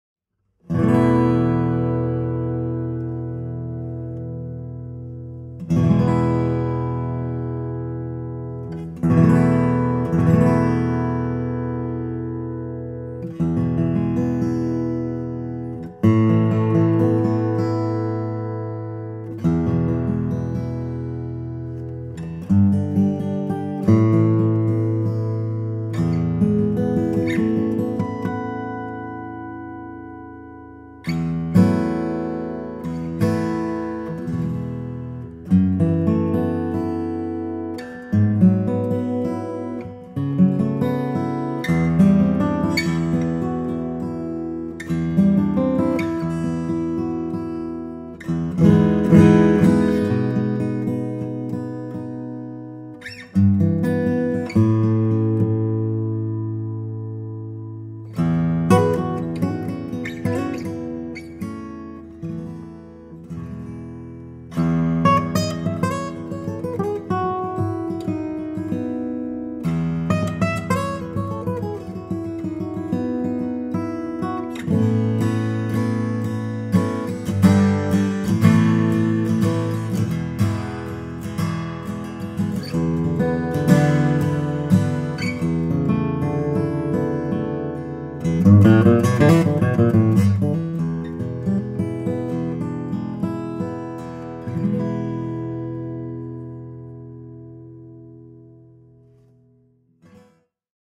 Martin D-35 Dreadnought: Acoustic Centre
MARTIN D-35 DREADNOUGHT. Best enjoyed with headphones or good speakers!

Known for its strong bass response, the D-35 acoustic Dreadnought guitar features a three-piece back and a bound fingerboard. The D-35 adds a Sitka spruce top to the East Indian rosewood back and sides. It's the guitar of choice for bluegrass and folk music. Perfect for the intermediate and advanced player. Comes complete with a Martin hard case.